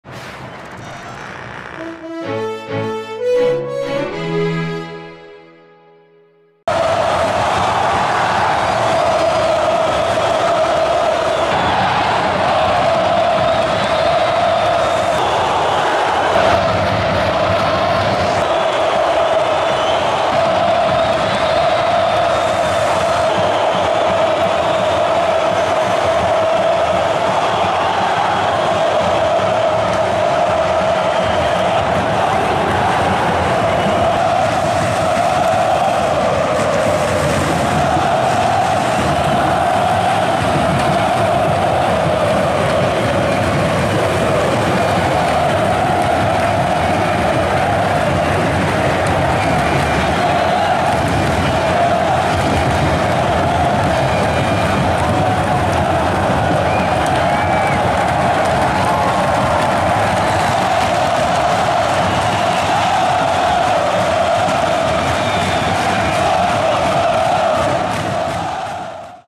Maior bandeirão do Engenhão!
A belíssima festa que a torcida fez no último domingo, contra o Flamengo!